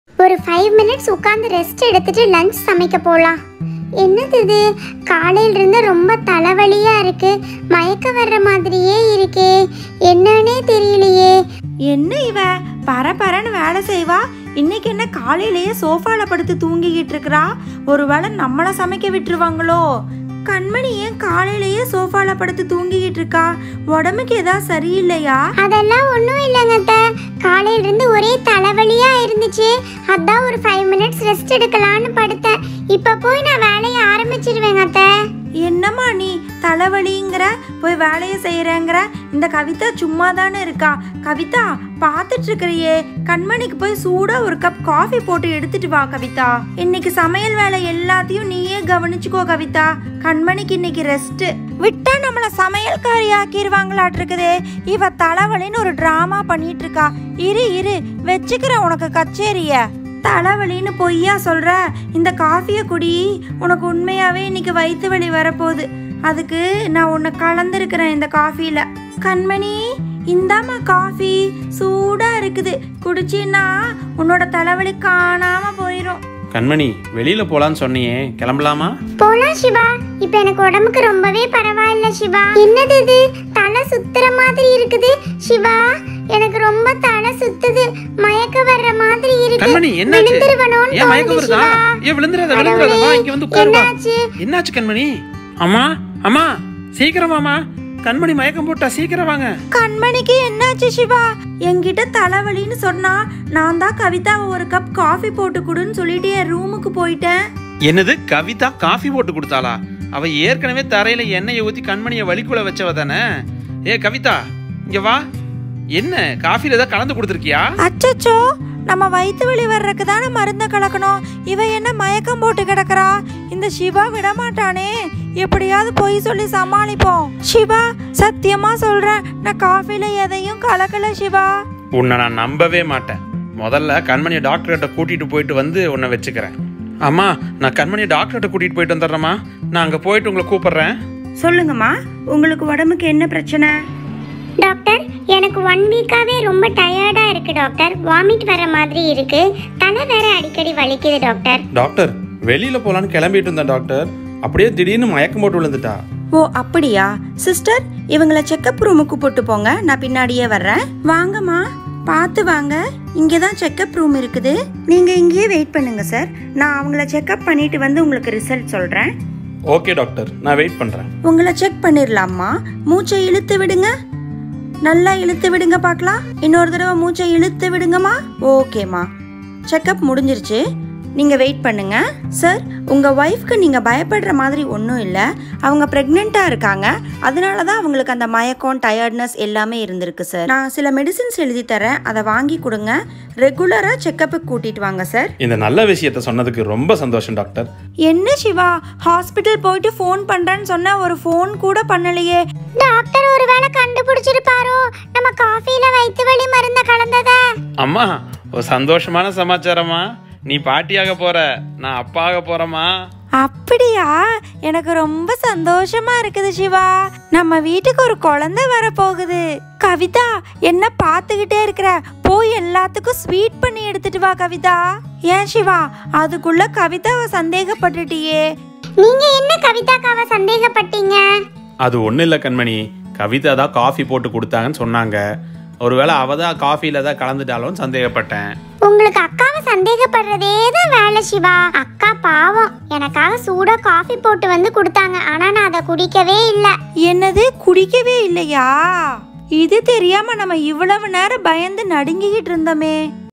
மண்வாசனை -Episode 108 | கண்மனியின் மயக்கத்திற்கு கவிதா போட்ட coffee தான்காரணமா ? | Classic Mini Food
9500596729 Is the WhatsApp number

00:00  மண்வாசனை - Episode 108 | கண்மனியின் மயக்கத்திற்கு கவிதா போட்ட coffee தான்காரணமா ? | Classic Mini Food

03:53 Recommended Video